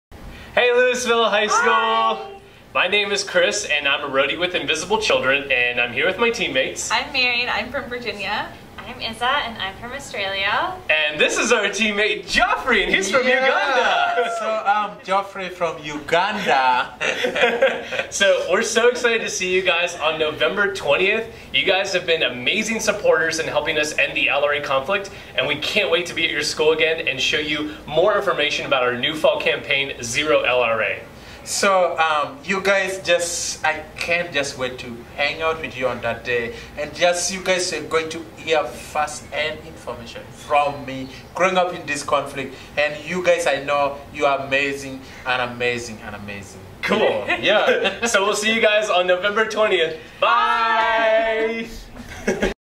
Invisible Children is Coming to Lewisville High School
We're so super stoked to see you guys on November 20th, and finally bring the LRA conflict to an end!!